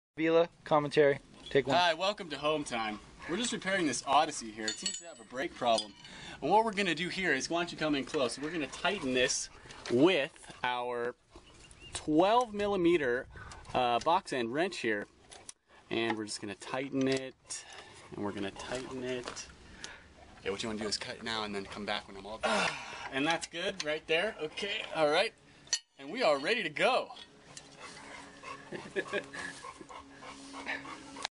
Les putting on a show, Vila style
One of the Odyssey wouldn't stop so our resident mechanic took a wrench to it